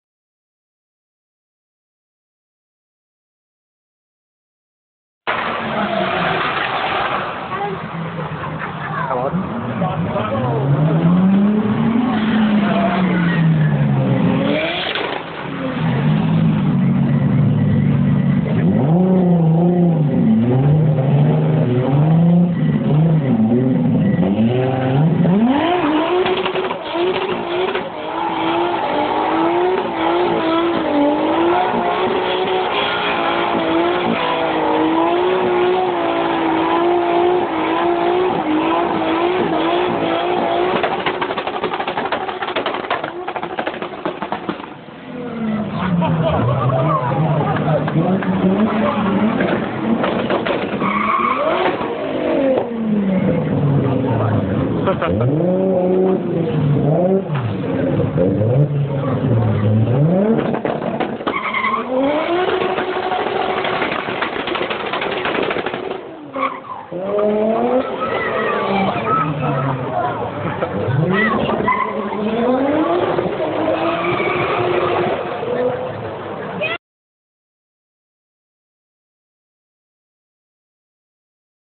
2 Toyota supra's huge burnouts
Sweet toyota supra's gave the best thay had